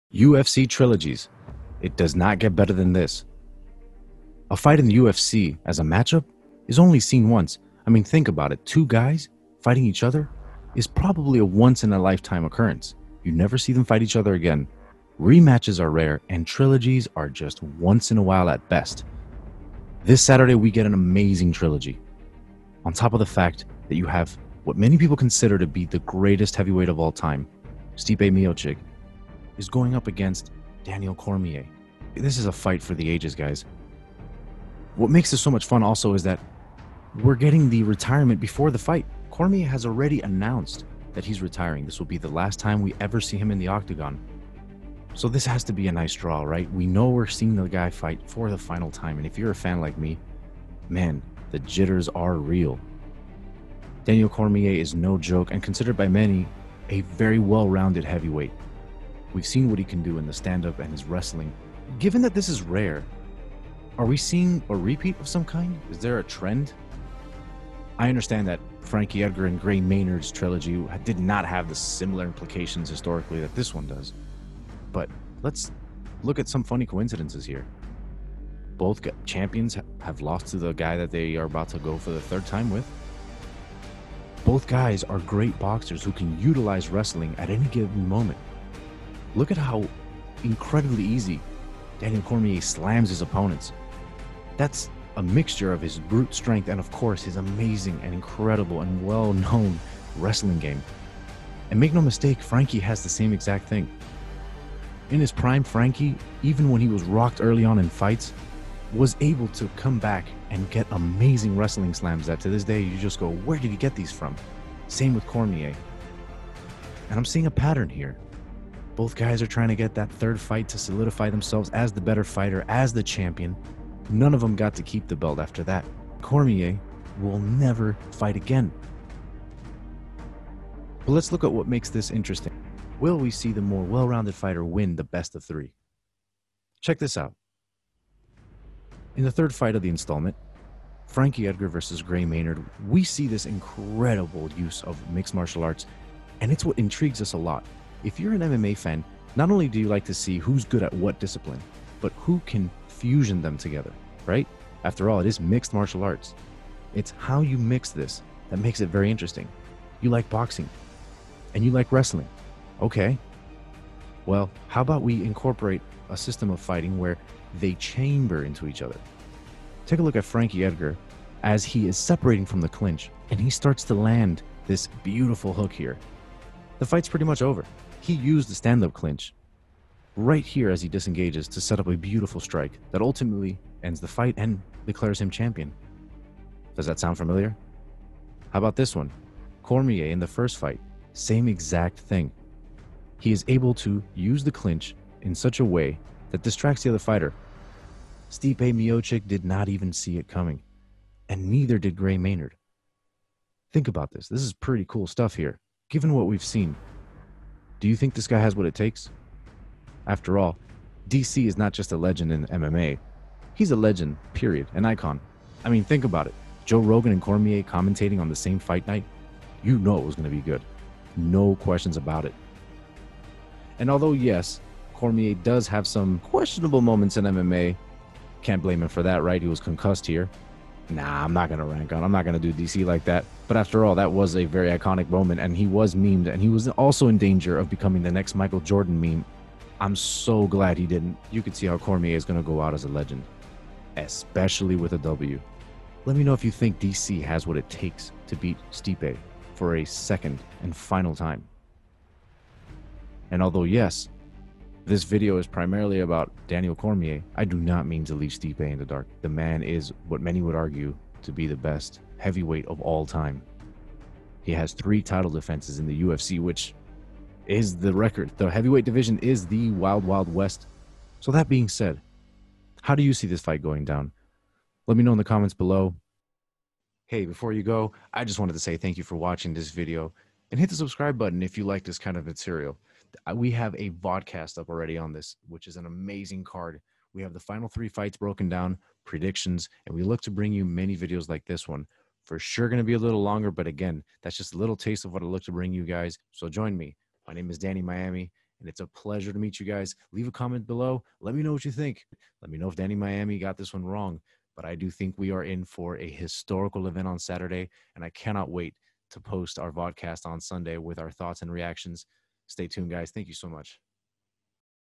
WILL UFC HISTORY REPEAT ITSELF THIS SATURDAY? (252 MAIN EVENT)
Trilogies are a rare thing in the UFC. As rare as they are, we have an odd similarity with DC's knockout in a Championship Trilogy when you look at Frankie's Knock out on Gray Maynard.
Will the legend Daniel Cormier solidify himself as one of the G.O.A.T.S?